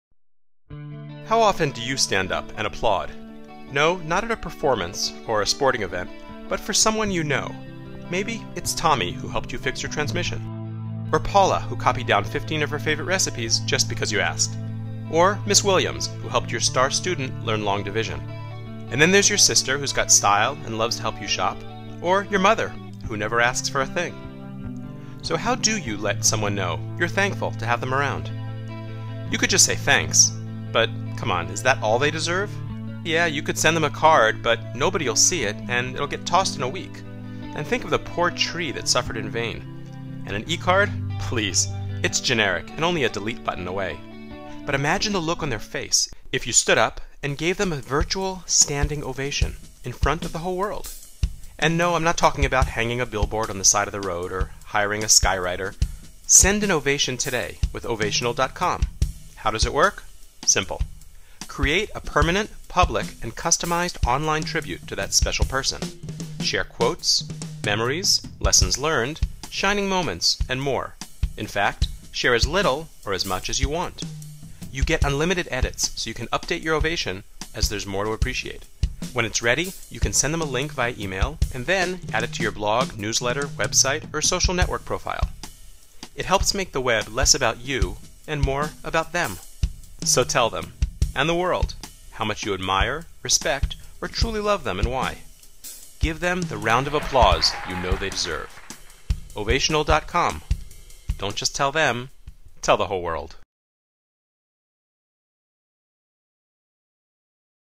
Stand up and Applaud with Ovational.com
An Ovation is a permanent, public, and customized online tribute to a special person who has significantly touched your life. It's the way to surprise a grandmother, boyfriend, teacher, colleague or even a favorite blogger by telling them -- and the world -- how much you admire, respect or truly love them and why.